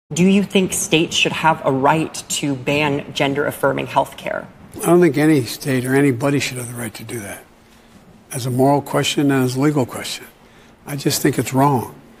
The White House invited Dylan Mulvaney- the transgender activist
The White House invited Dylan Mulvaney- the transgender activist who went viral for documenting “days of girlhood”